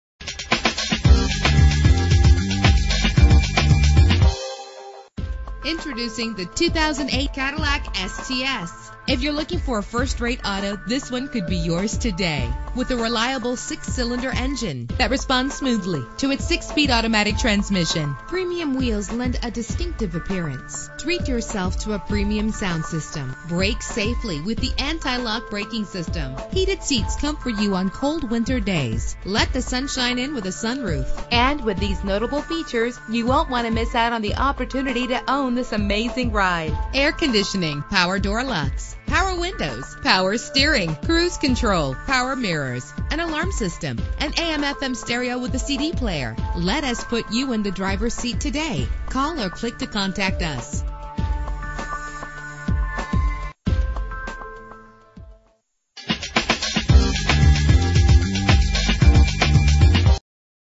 2008 Cadillac STS - Fairborn OH
Year: 2008
Make: Cadillac
Model: STS
Trim: V6
Engine: 3.6 liter 6 cylinder 24 valve
Transmission: 6-Speed Automatic
Color: Red
Mileage: 30205
Address:
1105 N Central Ave
Fairborn, OH 45324